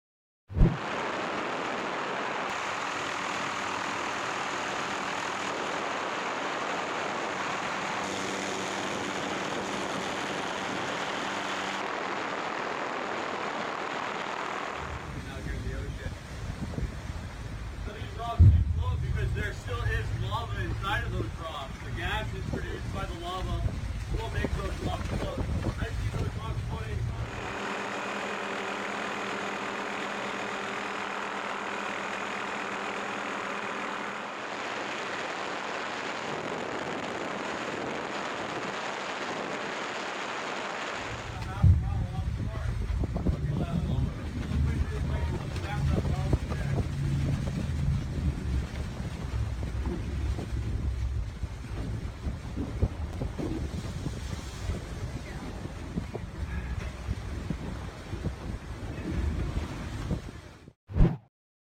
Deadly acid cloud rises over Hawaii as lava streams into ocean
The eruption of Kilauea volcano in Hawaii sparked new safety warnings about toxic gas on the Big Island’s southern coastline after lava began flowing into the ocean and setting off a chemical reaction.